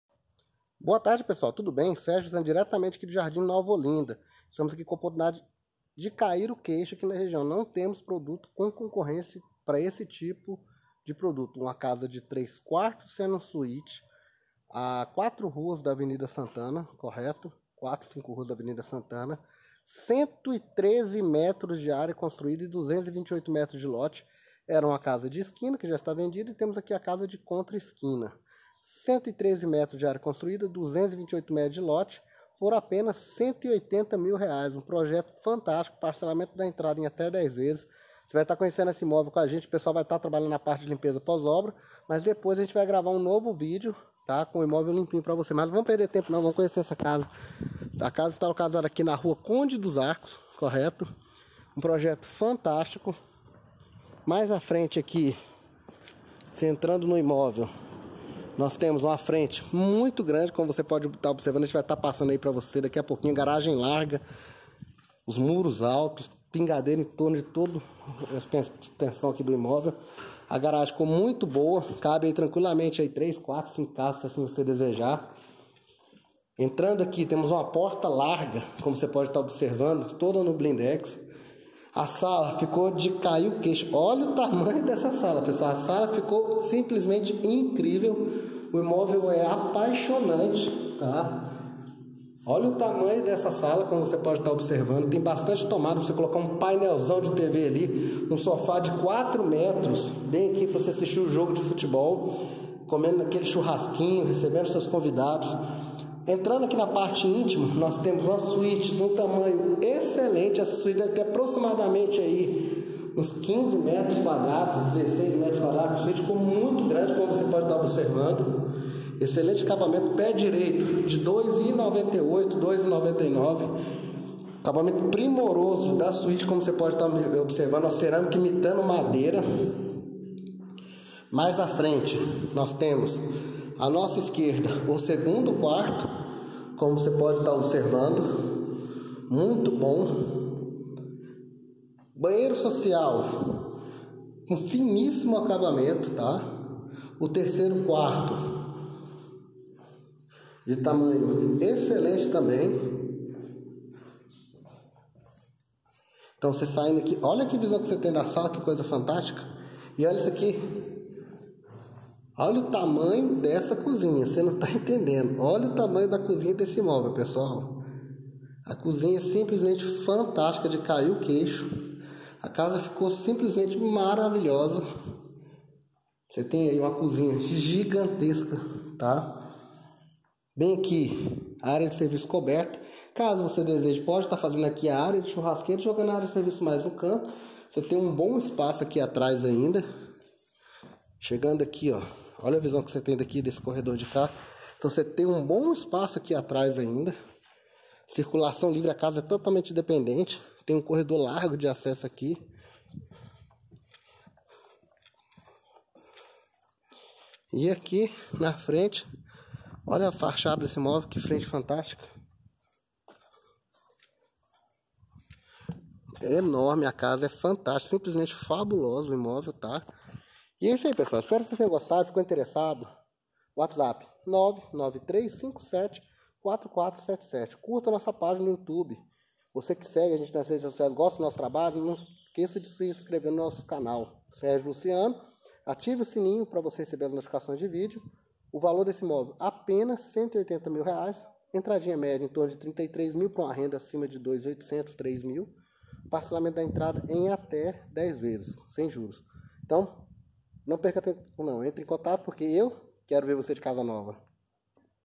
Magnífica casa de 3/4 sendo 1 suite , setor nova Olinda Aparecida de Goiânia GO
Casa de  3/4 com suíte 103 m , setor Nova Olinda , em Aparecida de Goiânia

Apenas R$ 180.000,00 lote de 220 m contra esquina

Casa em construção término com 15 dias

Imagens da casa

Ao lado do novo eixo NS1, próximo da BR 153 e a 7 min do Centro de Aparecida e do novo Shopping - sala ampla e arejada

- janelas grandes em Blindex
- quartos grandes
- cozinha padrão
- excelente acabamento
- quintal Grande

Casa próxima a avenida principal do setor ,com escolas, comércio , posto de saúde e linha de ônibus a menos de 300 m .

Entrada parcelado em até 10 x sem juros
Utilize seu FGTS , renda ideal a partir de 2.800,00

Agende hoje mesmo sua visita

Sérgio Luciano
Creci 18246